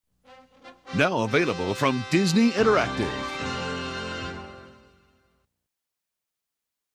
Now Available From Disney Interactive Zoom-in Bumper [Fanmade, Extremely Rare!!!]
This is a Fanmade Zoom-in Bumper of “Now Available From Disney Interactive” Bumper.